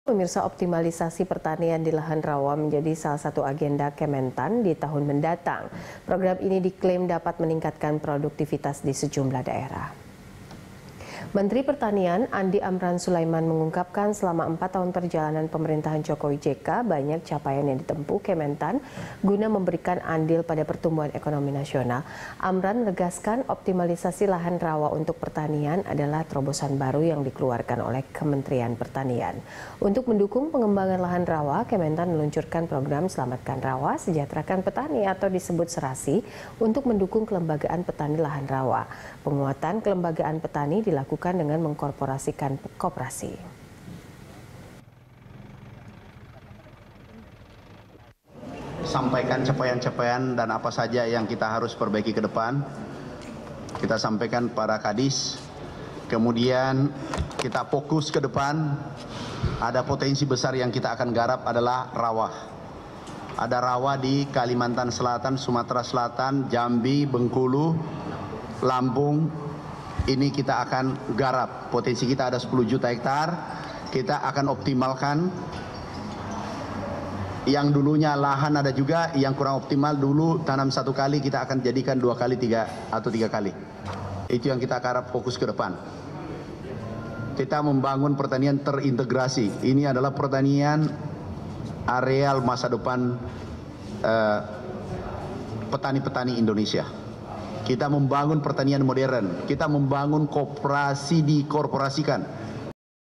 2019, Kementan Optimalisasi Pertanian di Lahan Rawa
Optimalisasi pertanian di lahan rawa menjadi salah satu agenda Kementan di 2019. Program ini diklaim dapat meningkatkan produktivitas di sejumlah daerah.

Facebook.com/BeritaSatuTV
Youtube.com/BeritaSatu